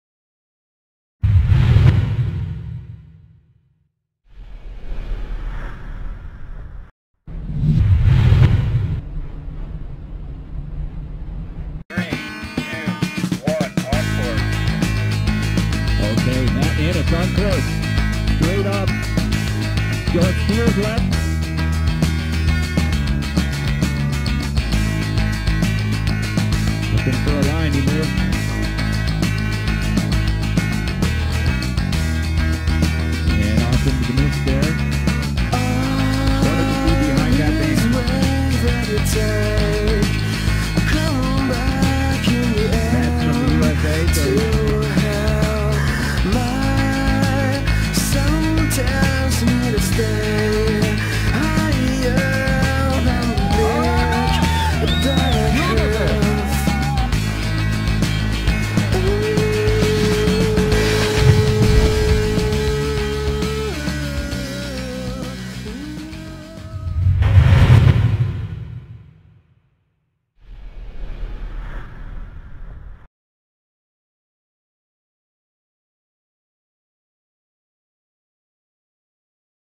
MATT ANNETTS (USA) - 2nd PLACE RUN - NISSAN TRAM FACE 2010
Matt took second at the Nissan Tram Face.